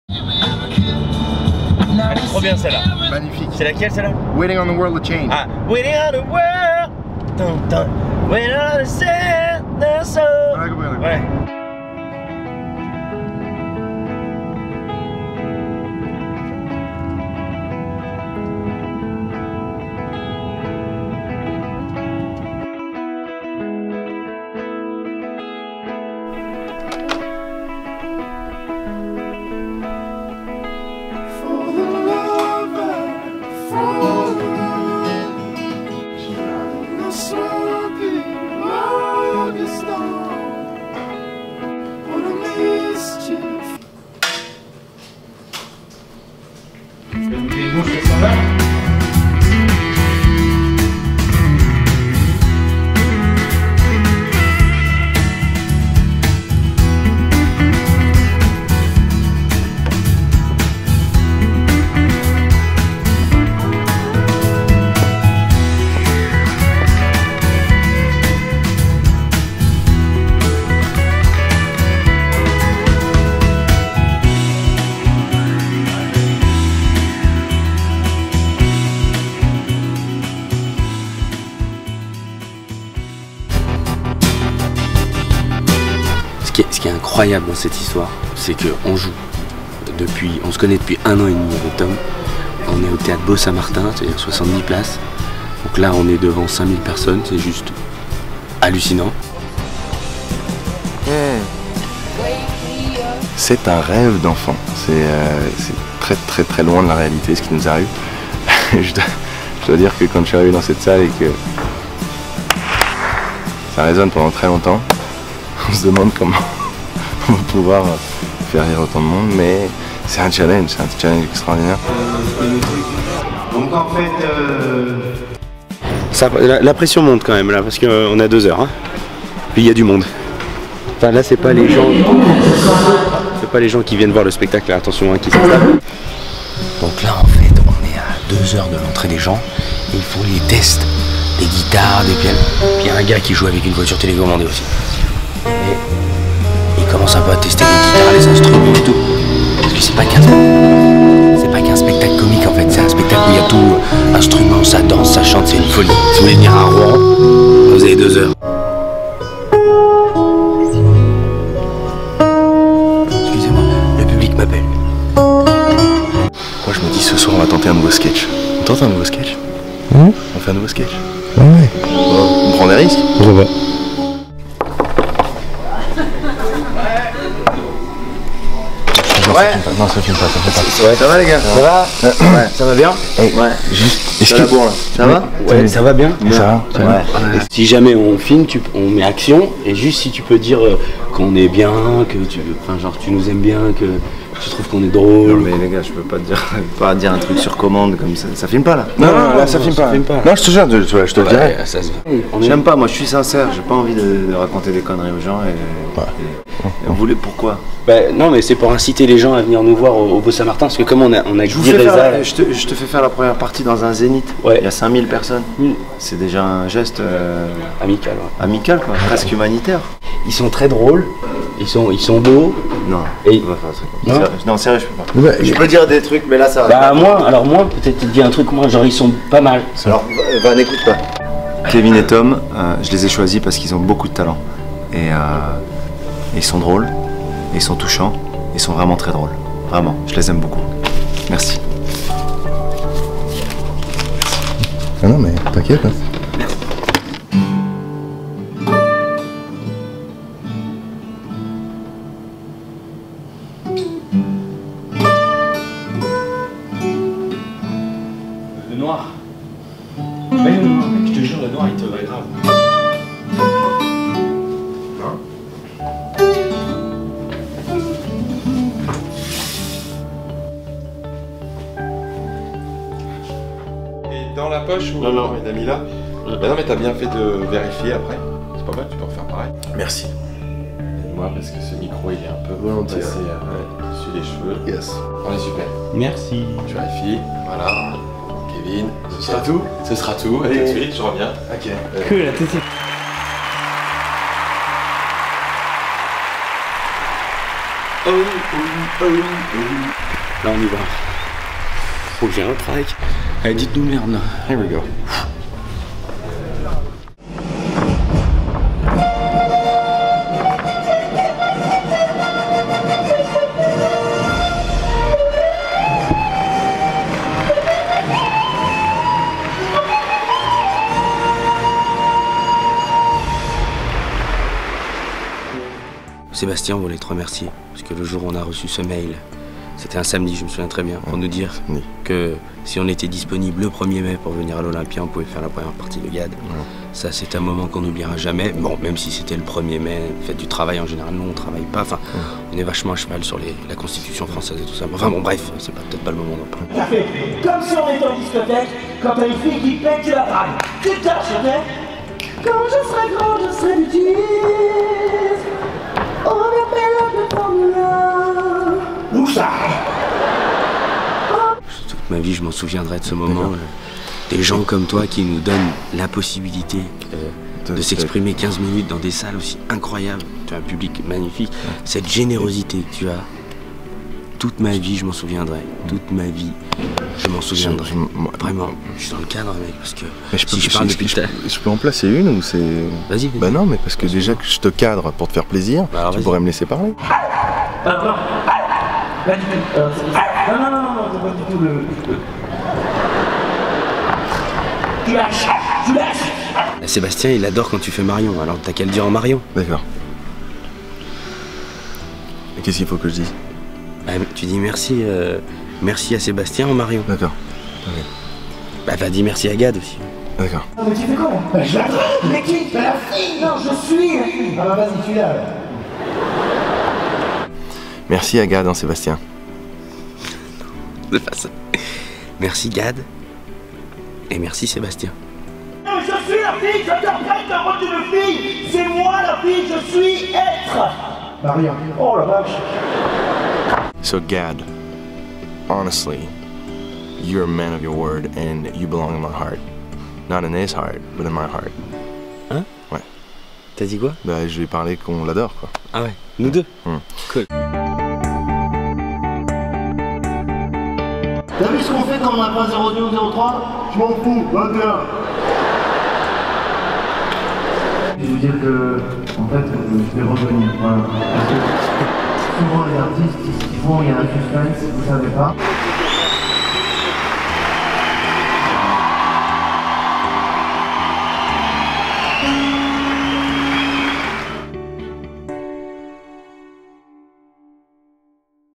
Gad Elmaleh : 1è partie avec Kevin & Tom ! (making of)
Kevin & Tom dans les coulisses de la 1ère partie de Gad Elmaleh au Zénith de Rouen !